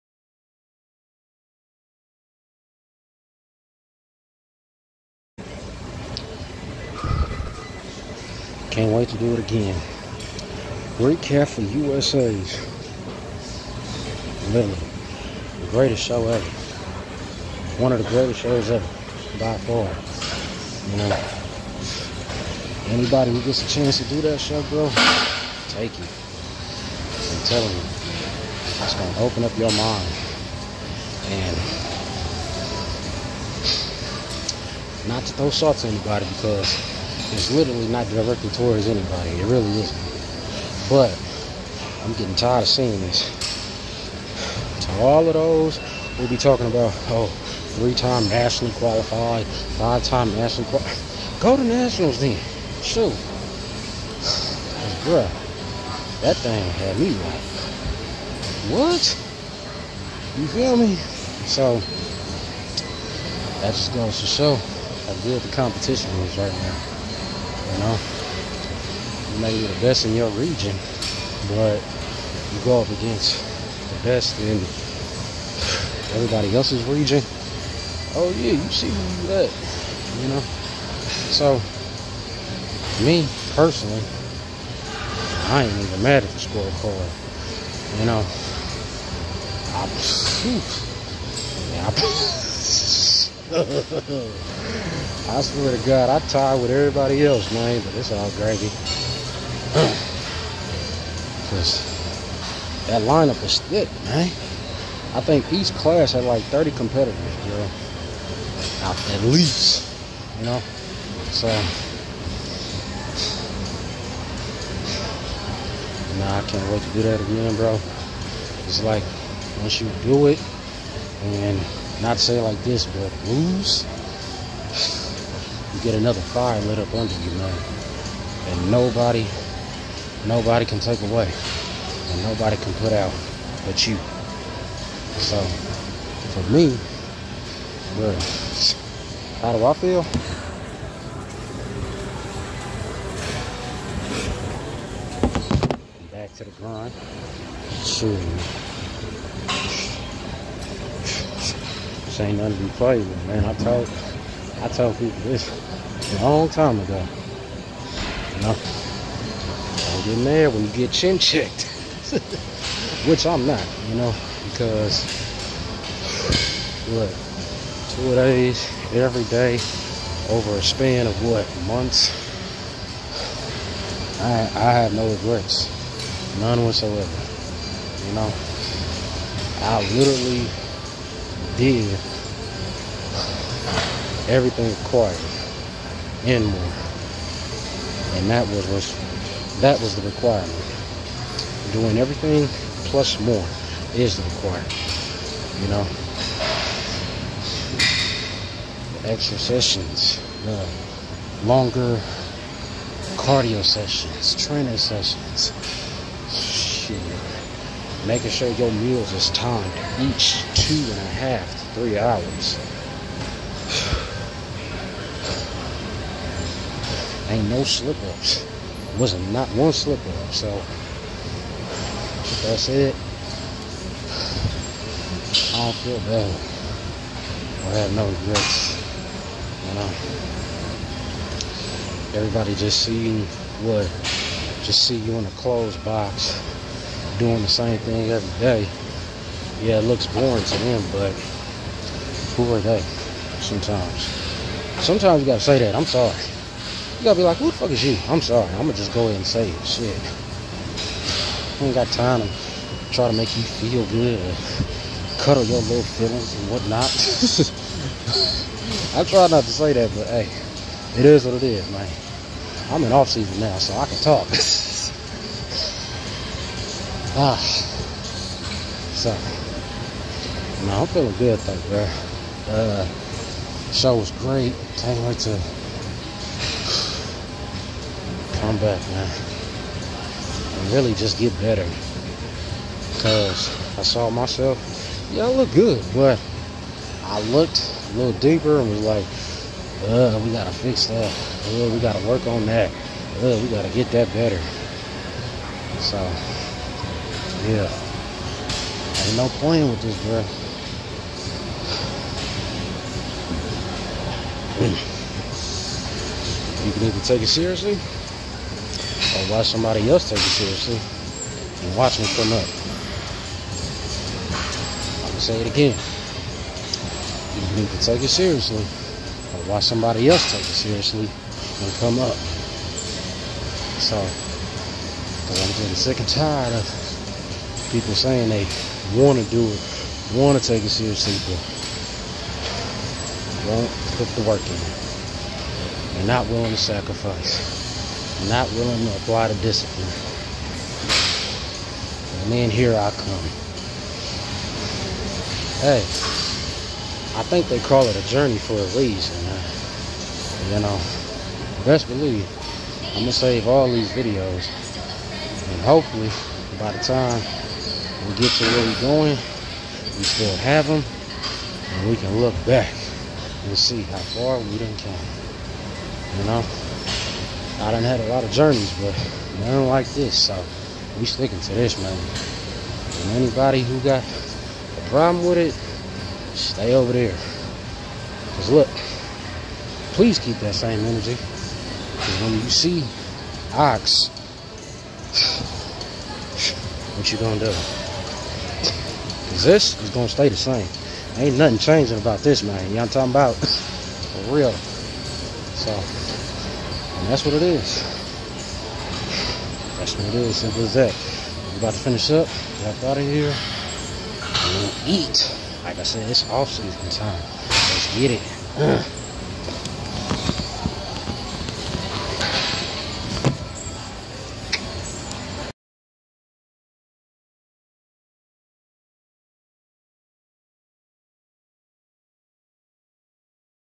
USA NATIONALS RECAP | Here's the outcome!
USA NATIONALS RECAP!!

Literally this was my best show for so many reasons. Once again this was a big eye opener for me to see where I rank against the best in the nation. Glad I was able to see it threw and go perform at one of the most prestigious shows there is. Such a blessing, but now it's back to work!! Still not slowing up no time soon. Watch the comeback!!!

SILENT MOVES = LOUD RESULTS!!!